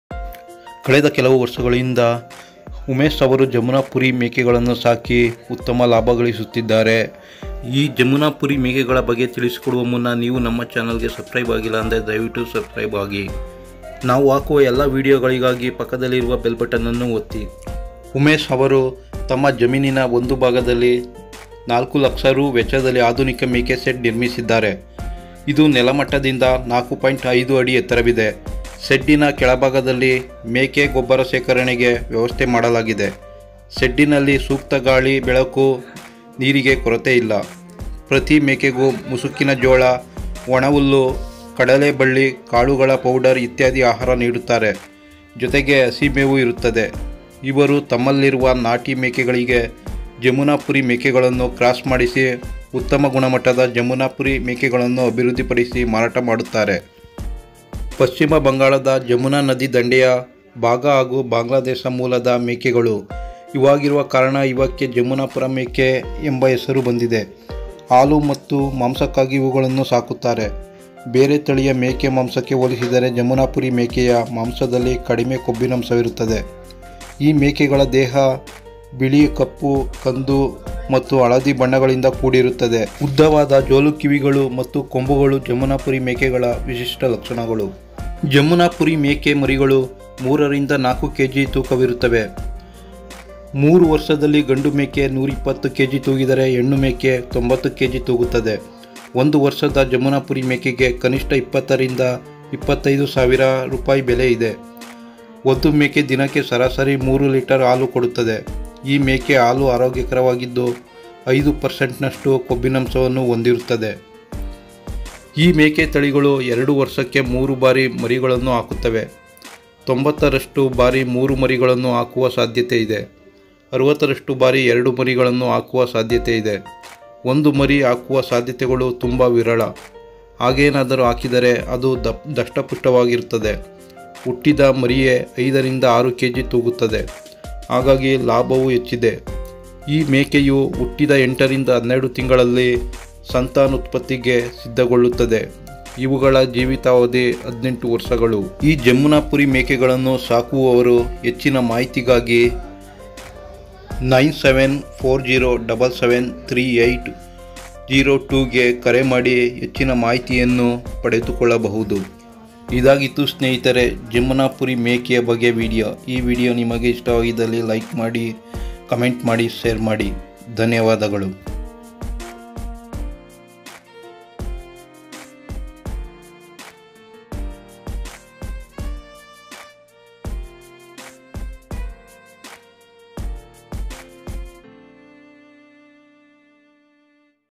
ಜಮುನಾಪುರಿ ಮೇಕೆ ಮಾಂಸಕ್ಕೂ ಸೈ ಹಾಲಿಗೂ ಸೈ. goatfarming jamunaparigoatfarming goatsale makesakanike.
120 kg tuguva jamunapuri make manda mattu halige upayoga.